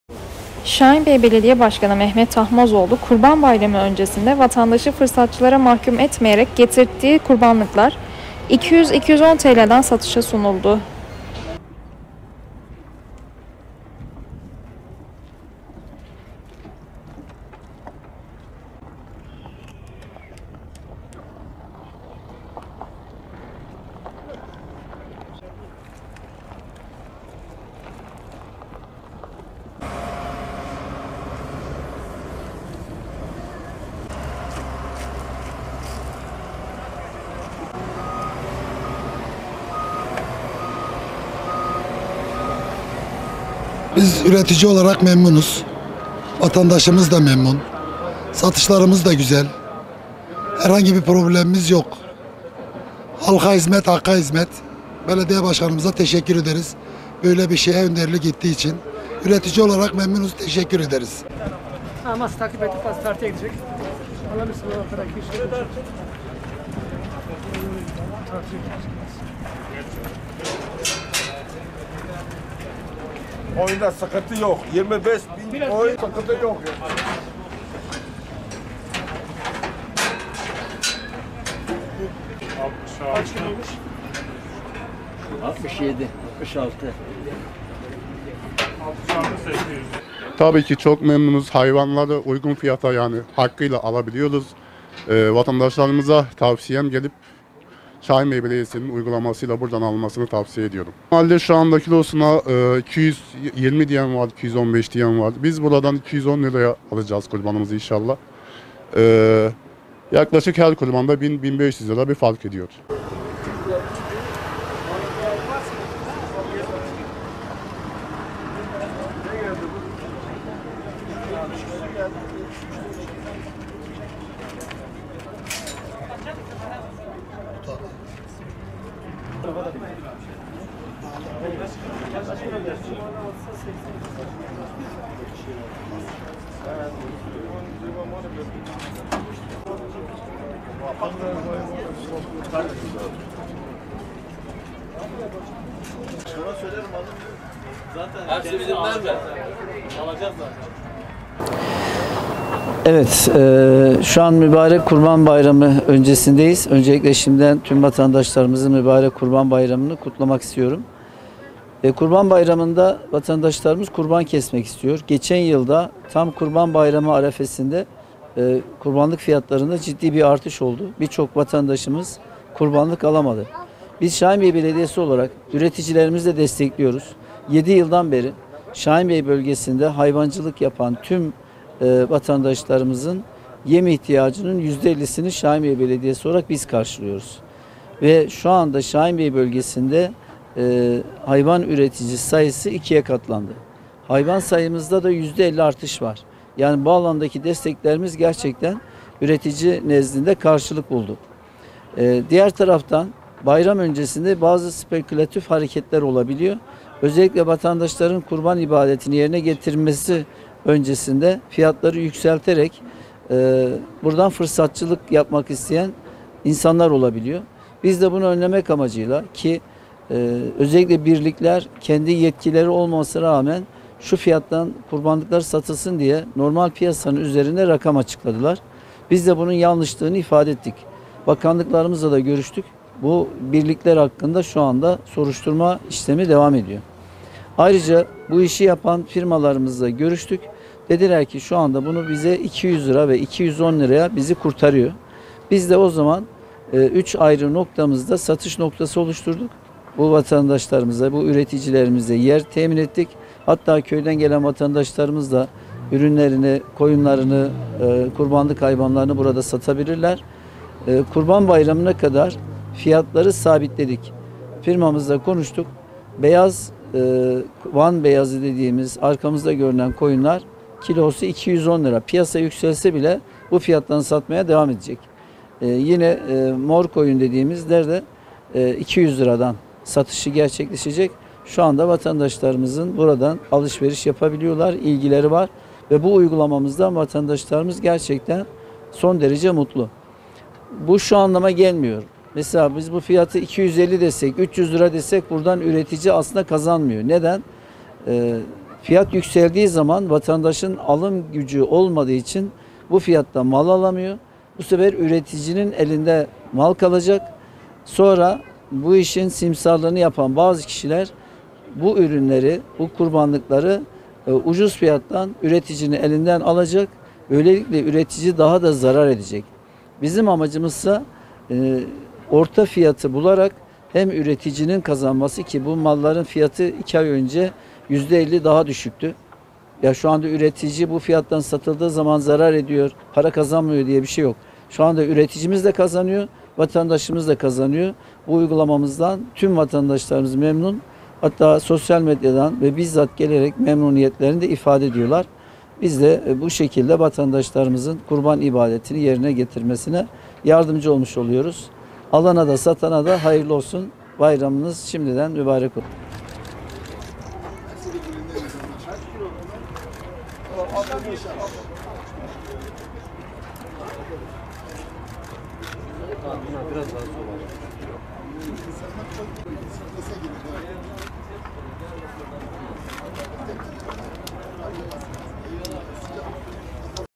Şahinbey Belediyesinden Kurbanlık Uygulaması
Şahinbey Belediye Başkanı Mehmet Tahmazoğlu, yüksek kurban fiyatlarına karşı önemli bir adım atarak vatandaşların bütçesini düşünen bir uygulama başlattı.